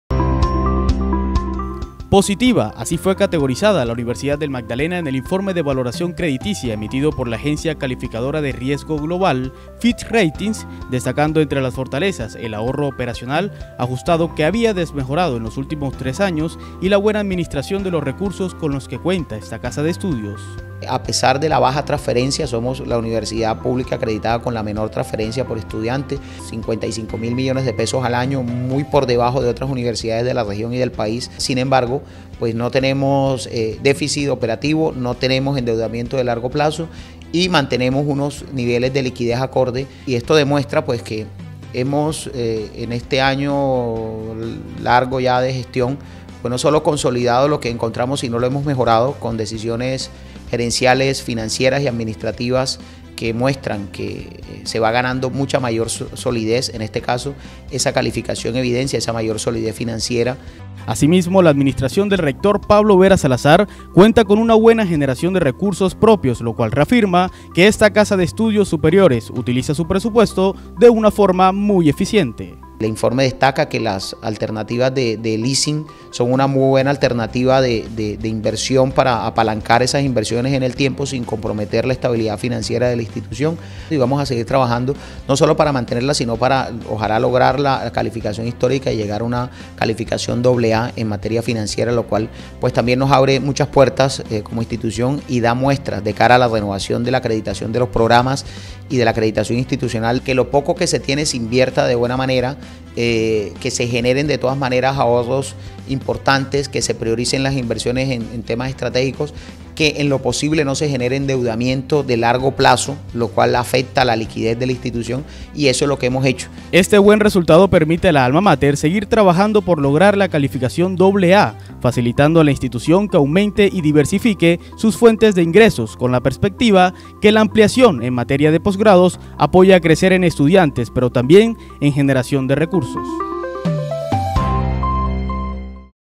Boletín Audiovisual 080 UNIMAGDALENA escala a Positiva en calificación por Fitch Ratings
Debido al ahorro operacional y el buen manejo del presupuesto, las directivas de la Universidad del Magdalena, lideradas por el Doctor Pablo Vera Salazar, recibieron oficialmente el informe de valoración crediticia emitido por la Agencia Calificadora de Riesgo Global, Fitch Ratings, en la cual se pasó de Estable a Positiva la Perspectiva de la calificación.